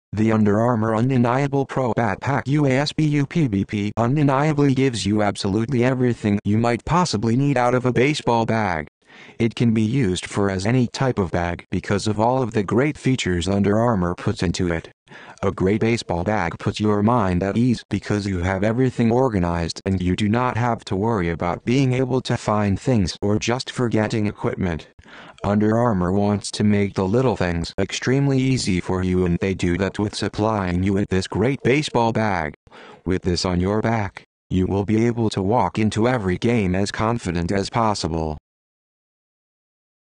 Under Armour Undeniable Pro Bat Pack UASB-UPBP | Baseball Bargains
The Under Armour Undeniable Pro Bat Pack UASB-UPBP has a large space for your gear while also having 4 bat holders.